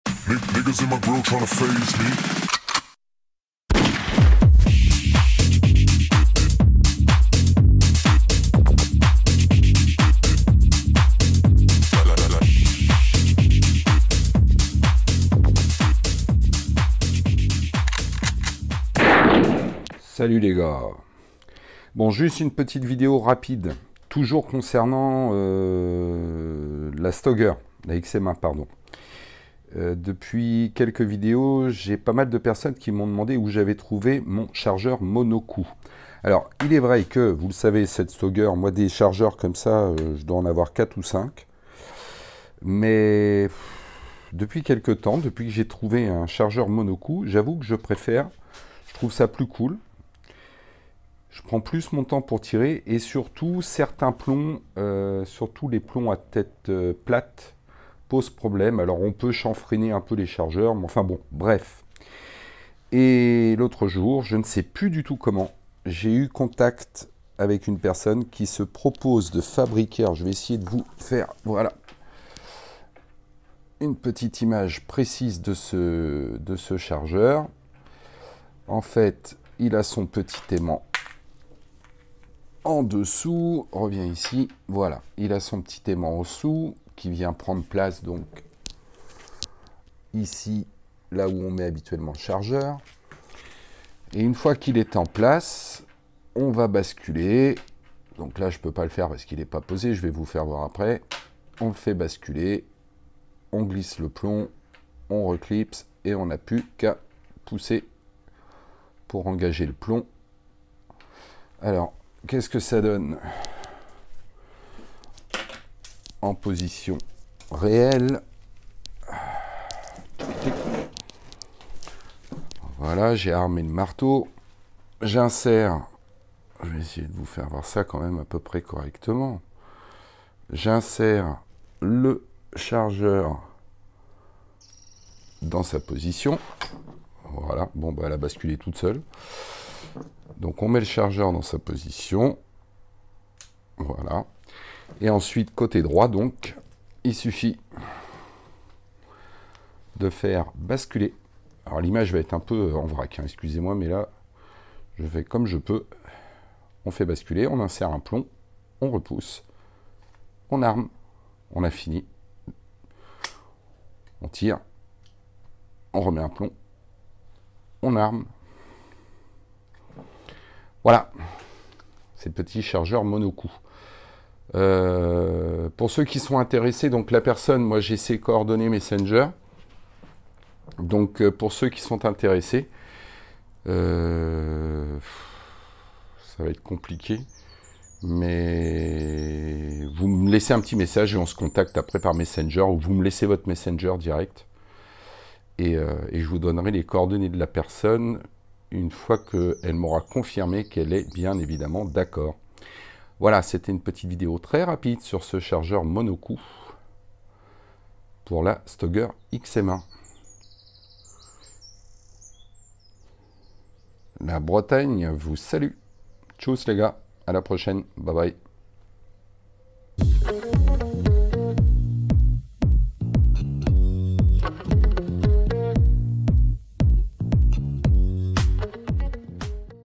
STOEGER XM1 Chargeur MONO
Présentation d'un chargeur Mono coup 3D pour la XM1